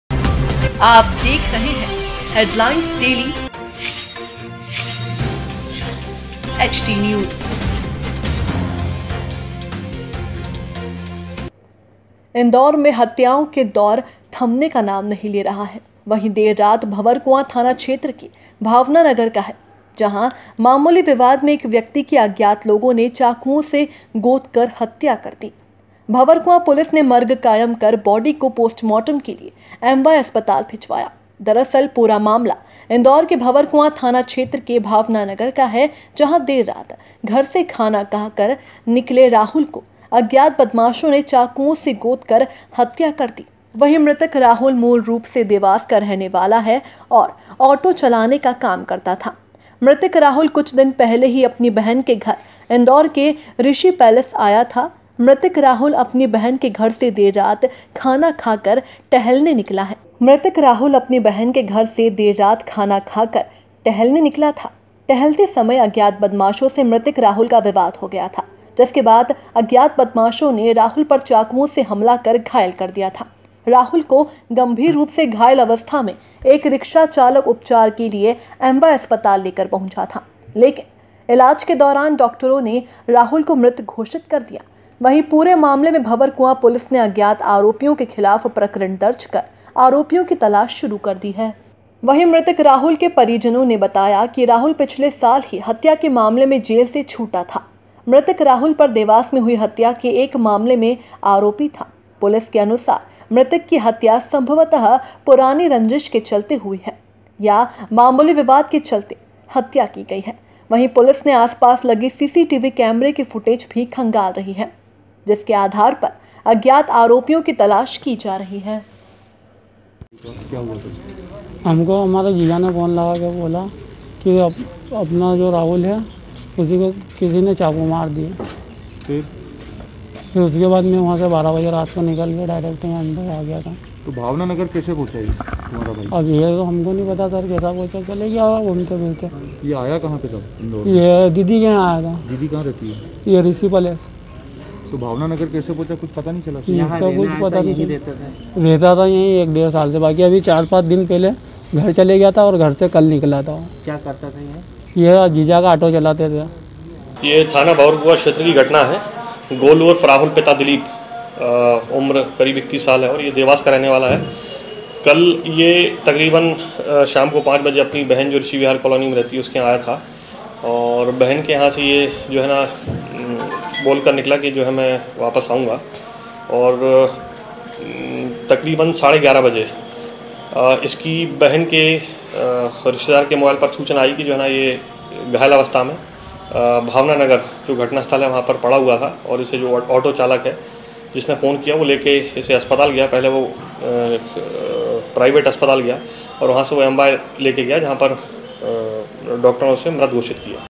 खाना खाकर टहलने निकले युवक की हत्या.. भंवरकुआं थाना क्षेत्र के भावना नगर में मामूली विवाद में
हुई युवक की हत्या